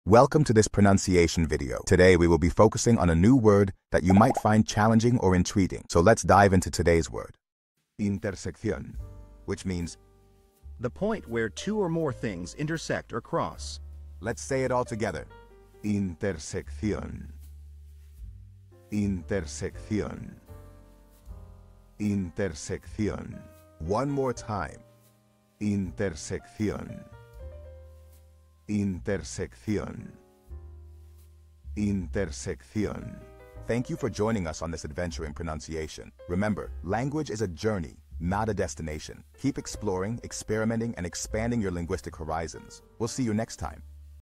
How to pronounce Intersección
Master the Pronunciation of 'Intersección which means intersection' - which means : The Point Where Two Or More Things Intersect or Cross. 📝 with @PronunciationSpanish 🎓🗣️ - [Your Guide to Spanish]

Learn to pronounce 'Intersección' perfectly in Spanish with our easy and engaging tutorial by @PronunciationSpanish! 📖📽️ In this comprehensive video, we provide you with native speaker examples 🗣️👂 and phonetic breakdowns 🎵 to ensure you get the pronunciation just right.

How to say Intersection in Spanish